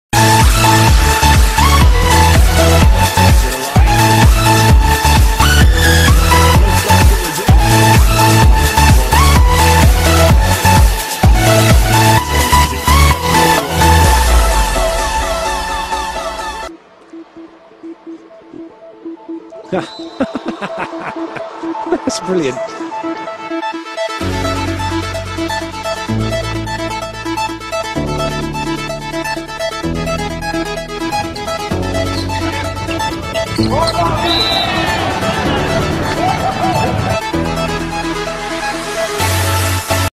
Chủ Công Trần Thị Thanh Thúy Tấn Công Sau Vạch 3m
Japan vs France - Highlights 2017 Men's World Grand Champions Cup Volleyball